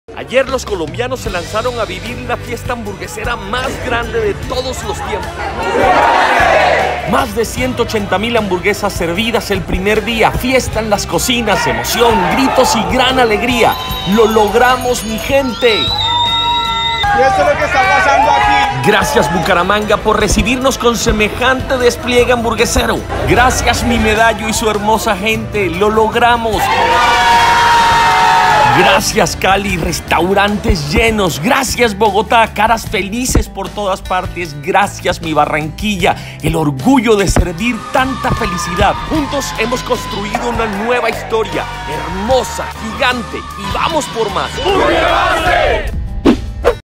¿Eres COLOMBIANO? Tienes que ver esto!!!
Ahora... prepárate para esto: Ayer los colombianos se volcaron a las calles a vivir la fiesta hamburguesa más grande de todos los tiempos: Burger Master!

Más de 180.000 hamburguesas servidas el primer día… fiesta en las cocinas, emoción, gritos de alegría: BRAVO!!! lo logramos mi gente!

GRACIAS a Bucaramanga por recibirnos con semejante despliegue hamburguesero! Gracias a mi Medallo y a su hermosa gente! Gracias a mi Cali bella… restaurantes llenos… gracias a mi Bogotá del alma… caras felices por todas partes… gracias a mi Barranquilla gozona, el orgullo de servir tanta felicidad.

Juntos hemos construido una nueva historia y vamos por más!

BURGER master NACIONAL
Hasta el próximo domingo 6 de Mayo, 180 hamburgueserías de Bogotá, Medellín, Barranquilla, Cali y Bucaramanga, lucharán a muerte por demostrar ¿quién tiene la mejor Hamburguesa de su ciudad? Podrás probarlas todas y pagar por cada una solo $10.000 pesos! Al final el jurado serás tú. Para saber ¿cómo votar y ganar?, y para encontrar las hamburgueserías participantes en tu ciudad, descarga la APP BURGER Map desde